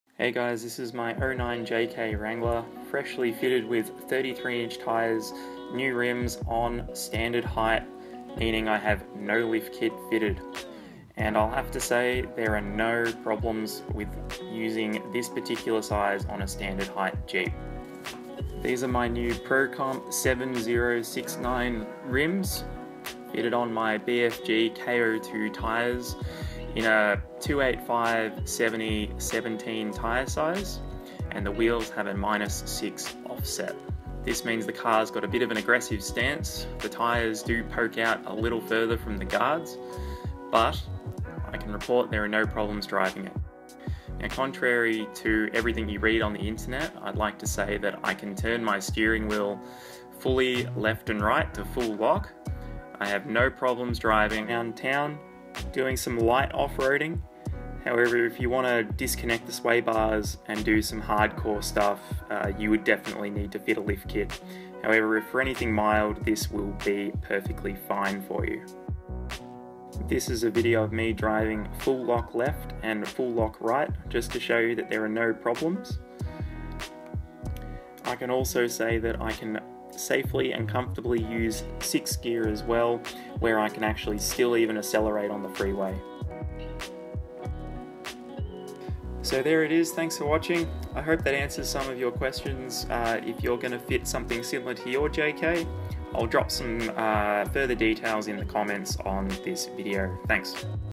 Will 33" Tires fit on a stock JK with no lift? - Jeep Wrangler
Recently fitted some shoes onto my Jeep JK Wrangler with no lift on 285/70/17 BFG K02 (33") Tires with 17" Pro Comp 7069 Alloy Wheels.  All the Details in Full Description Below:

TIRES: BFG K02 All Terrains - 285/70/17
Search for Tires
WHEELS: Pro Comp 7069 Matte Black Wheels - 17x9", 4.75" Backspace, -6 Offset.
Search for Wheels (check fitment for your car)

I had done a lot of research over the internet before fitting these tyres and heard all the warnings: That especially with a 3.8 motor that 6th gear wouldn't work, that even with aftermarket wheels there would be clearance issues and I would not be able to turn the wheels full-lock. So I put this quick video together to show that the Jeep still drives fine and looks great.

The jeep will still accelerate and gain speed when using 6th gear. Can turn full lock both left and right, and have no rubbing or scrubbing issues.

However, I would advise this only is a safe height for very light off-roading, definitely NOT rock crawling, and preferably for street driving only.
For those considering fitting rims and tires before fitting a lift kit - this is to show you can do it with 33's before fitting your lift.
*Video based on personal experience only, yours may vary. Cheers